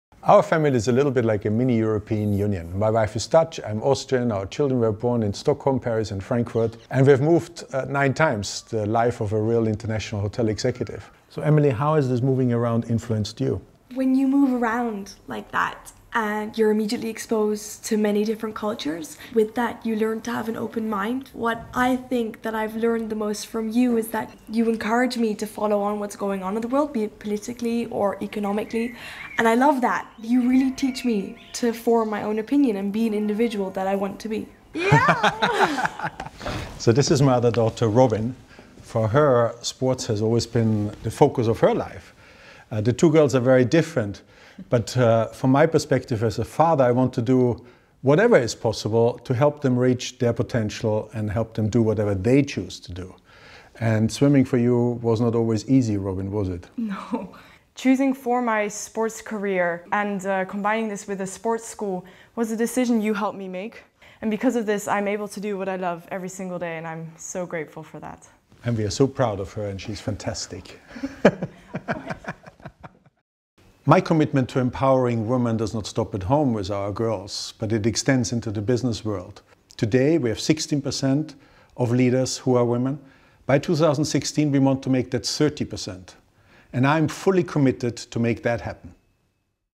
Wolfgang M. Neumann - Fathers Empowering Daughters
Watch Wolfgang M. Neumann, President & Chief Executive Officer of the Rezidor Hotel Group, sit down with his daughters and discuss the importance of Fathers Empowering Daughters as he joins our global campaign.

The goal of Fathers Empowering Daughters is to engage boys and men in the economic advancement and inclusion of women worldwide.